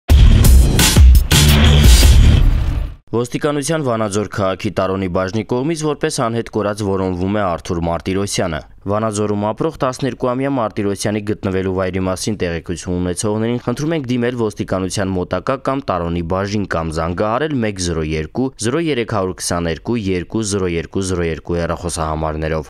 Անհետ կորած, աուտիզմով երեխան մահացած է հայտնաբերվել
Որոնողական աշխատանքների շնորհիվ նոյեմբերի 9-ին, ժամը 11.15-ի սահմաններում, Վանաձորի «Լճեր» կոչվող տեղանքում՝ լճի մեջ, հայտնաբերվել է որպես անհետ կորած որոնվող 12-ամյա Արթուր Մարտիրոսյանի դին, հայտնում է ոստիկանությունը։

Մանրամասները՝ տեսանյութում։